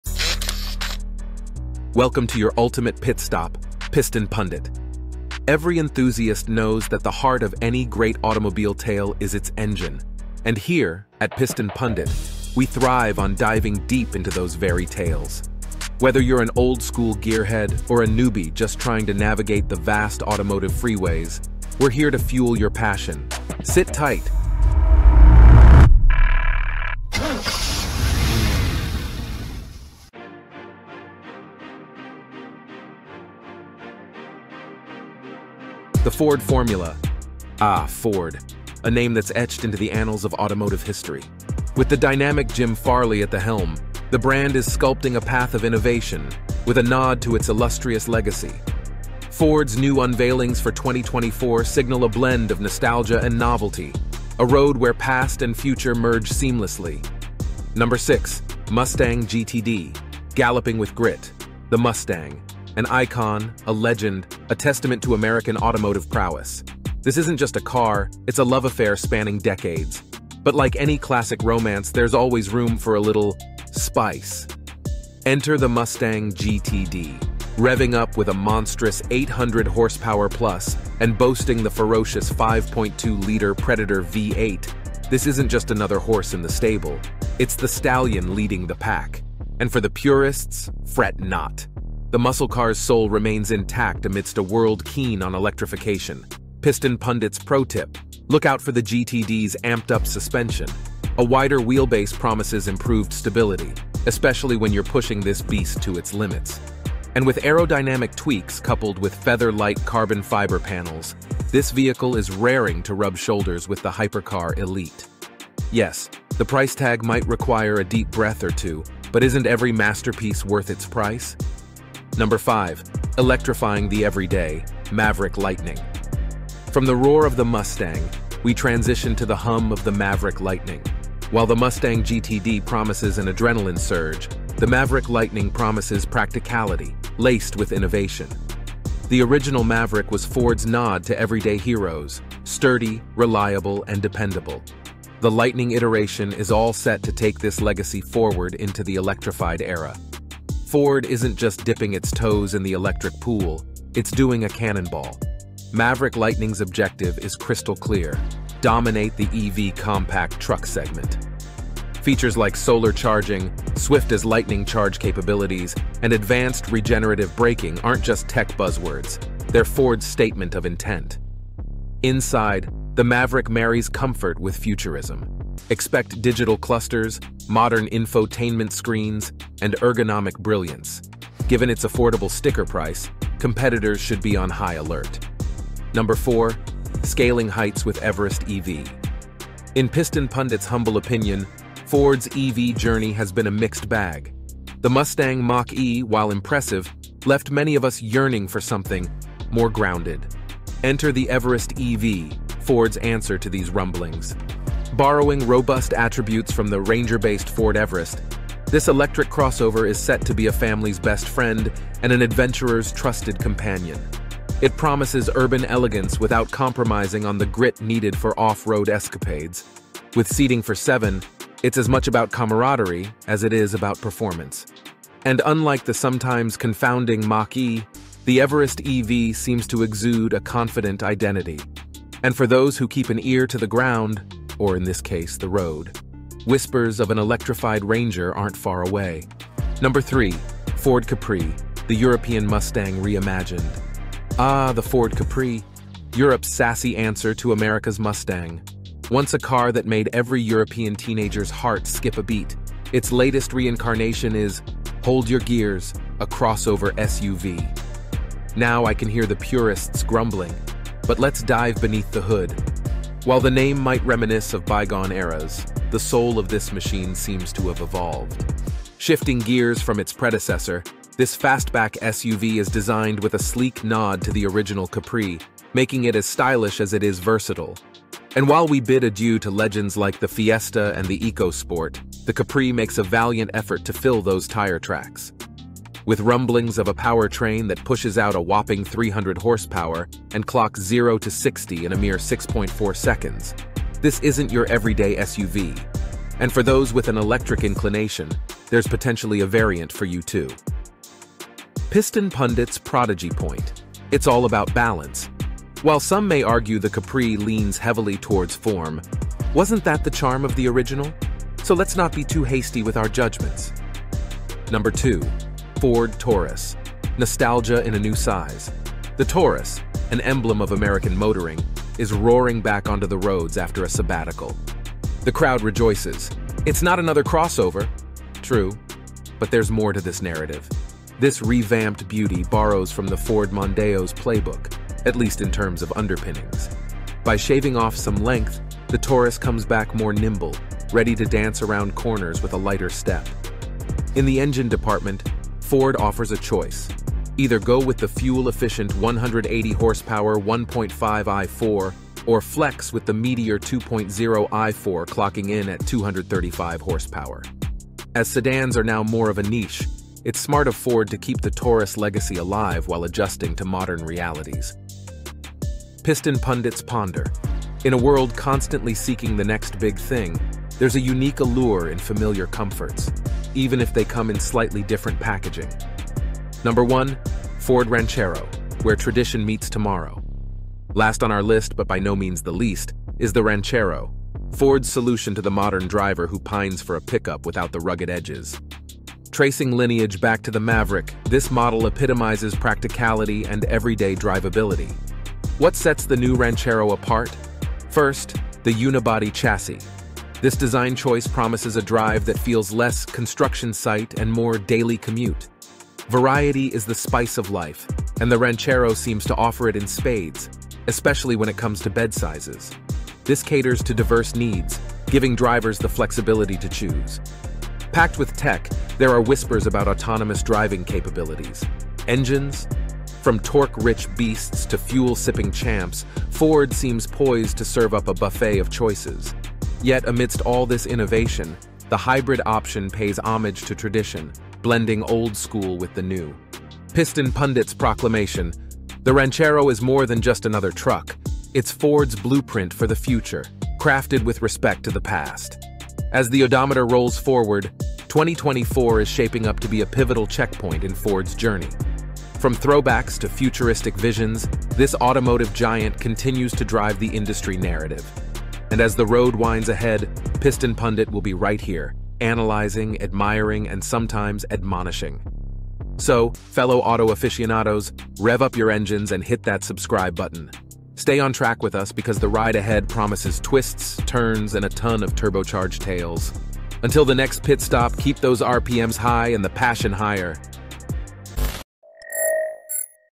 2024 Ford Lineup Revealed: CEO Unveils 6 Game Changers! | Piston Pundit
In this video, delve deep into Ford's new unveilings for 2024. Witness the seamless blend of nostalgia and novelty as the iconic brand crafts the future while honoring its legendary past. Prepare for a turbocharged ride with Piston Pundit!

*️⃣ 00:00 | Intro
🔎 00:30 | The Ford Formula
6️⃣ 00:57 | Mustang GTD: Galloping with Grit
5️⃣ 02:01 | Electrifying the Everyday: Maverick Lightning
4️⃣ 03:06 | Scaling Heights with Everest EV
3️⃣ 03:59 | Ford Capri - The European Mustang Reimagined
2️⃣ 05:14 | Ford Taurus - Nostalgia in a New Size
1️⃣ 06:16 | Ford Ranchero - Where Tradition Meets Tomorrow

Ford's 2024 line-up promises a blend of legendary throwbacks and future-forward innovations. Join Piston Pundit as we take a deep dive into the most anticipated vehicles from Ford's stable, offering expert insights, unique perspectives, and a dose of automotive passion. Don't miss out on this captivating journey!